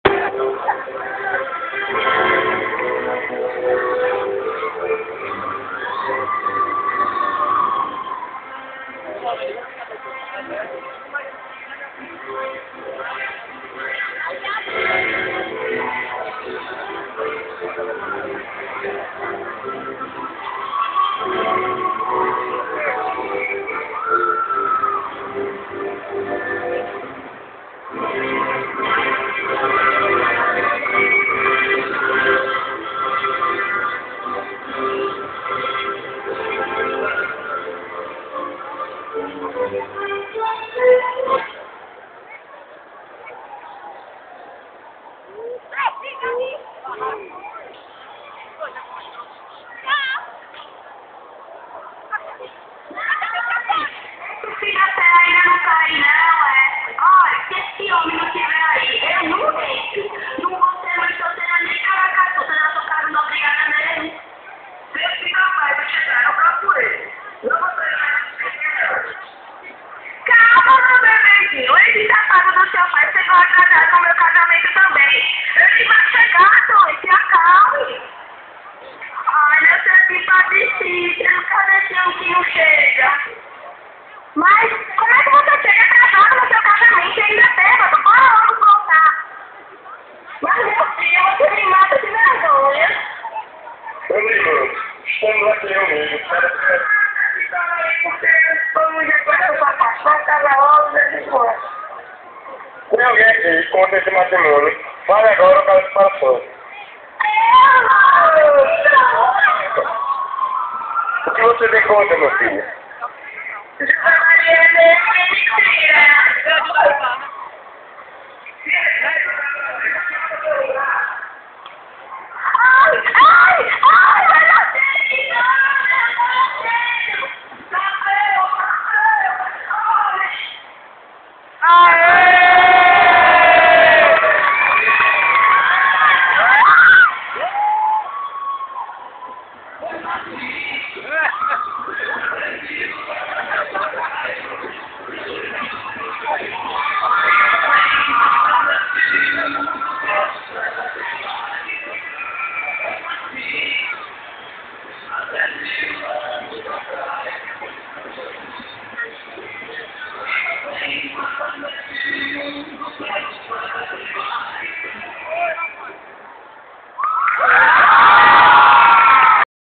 Quermesse Master - Casamento Caipira
Na Quermesse do Colégio Master, na prova do Casamento Caipira a quadrilha "Tia Lígia" fazendo uma homenagem a sua queriada professora de português, Lígia.